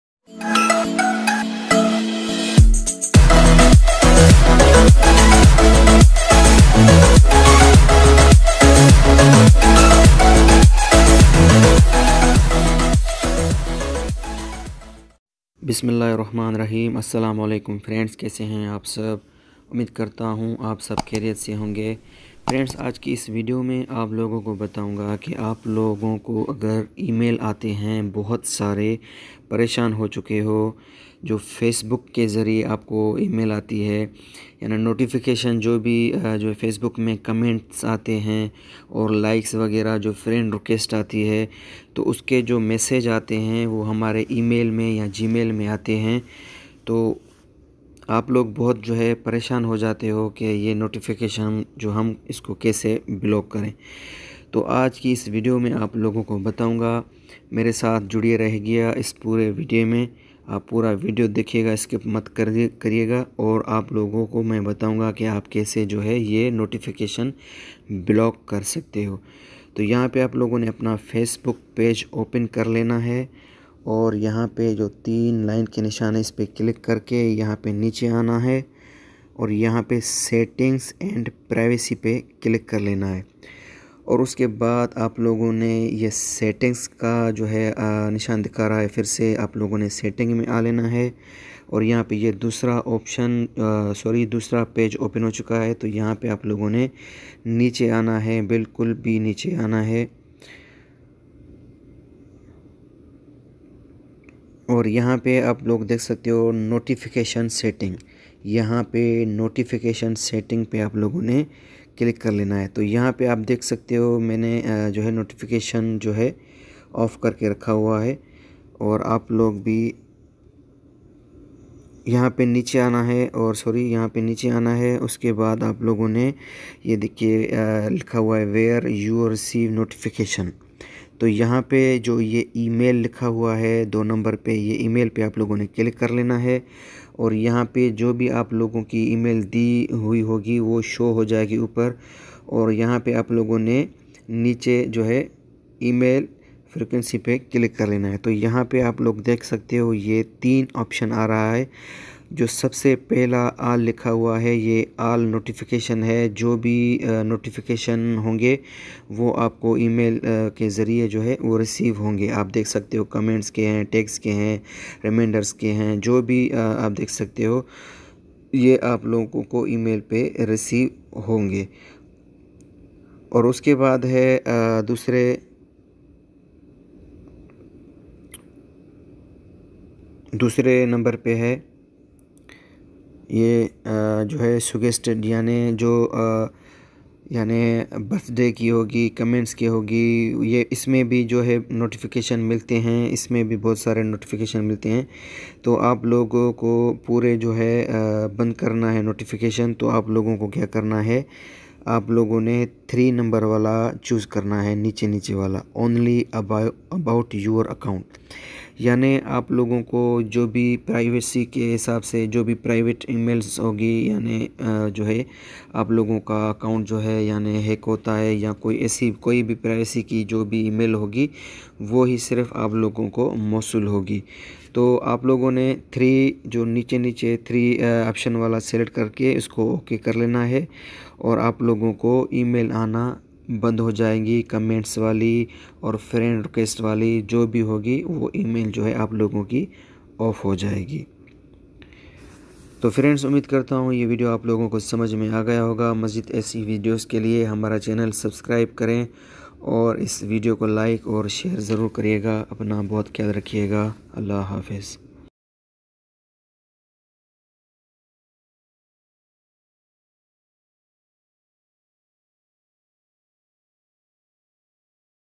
How To Stop And Block Facbook Notification In Email & Gmail,With Urdu
Asslam o alekum friend is video men aapko bataonga ke aap facbook ke notification email or gamil pe ana band kese karenge